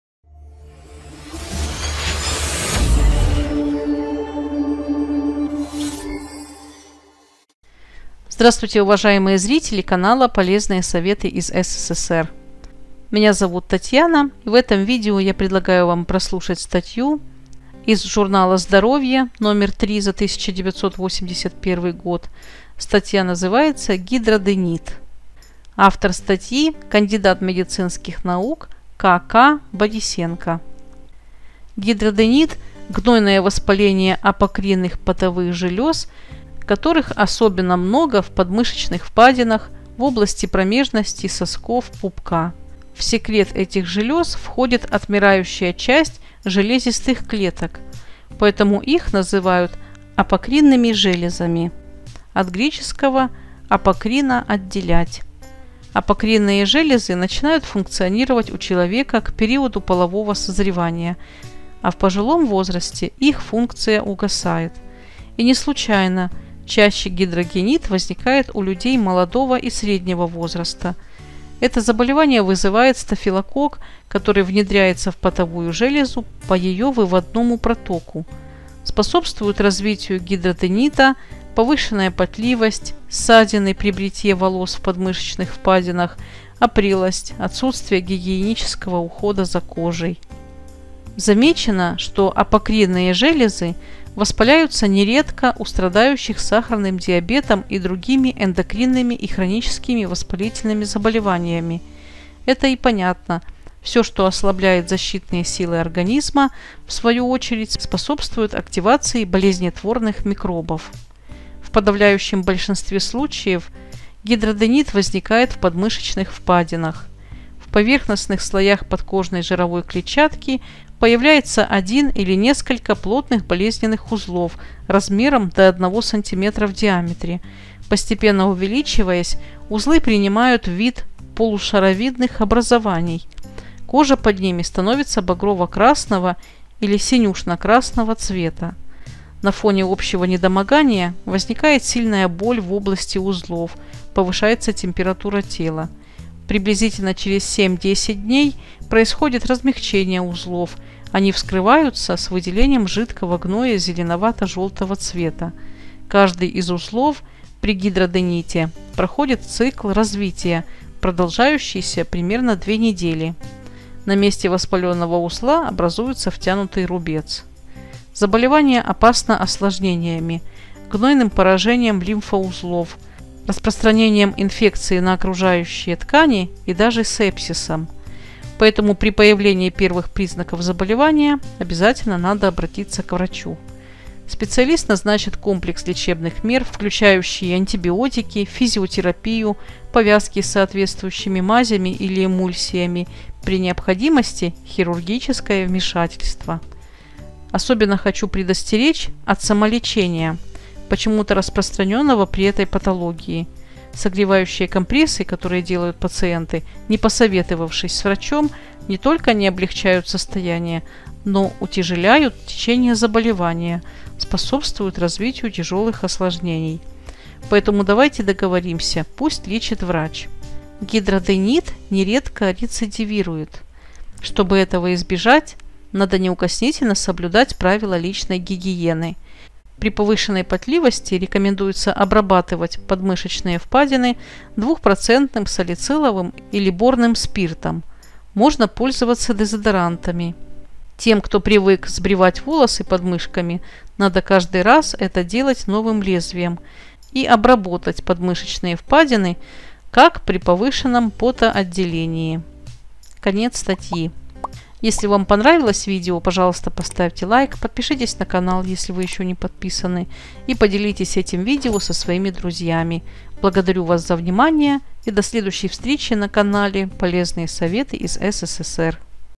Гидраденит под мышкой причины и лечение. Как лечить гидраденит в домашних условиях. Полезные советы
ГИДРАДЕНИТ

Статья из журнала Здоровье №3 за 1981 год.
Гидраденит — гнойное воспаление апокринных потовых желез, которых особенно много в подмышечных впадинах, в области промежности, сосков, пупка.
В секрет этих желез входит отмирающая часть железистых клеток. Поэтому их и называют апокринными железами (от греческого apokri-по—«отделять»). Апокринные железы начинают функционировать у человека к периоду полового созревания, а в пожилом возрасте их функция угасает. И не случайно чаще гидраденит возникает у людей молодого и среднего возраста...
Содержание видео:
1. Причины гидраденита
2. Стадии гидраденита
3. Лечение гидраденита
4. Профилактика гидраденита